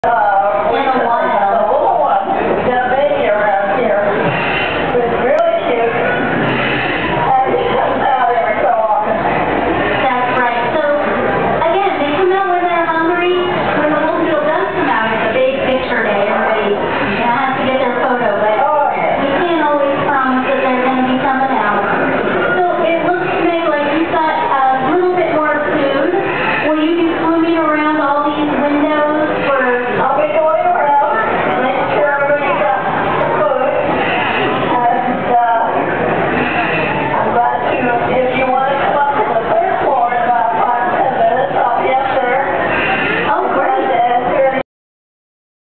Monterey Bay Aquarium Feeding Time
We watch as a diver went down the big aquarium equipped with an oxygen tank and food for all the fishes in the tank which includes a baby shark, 3 foot long. Seems like she was having fun feeding the creatures of that huge aquarium.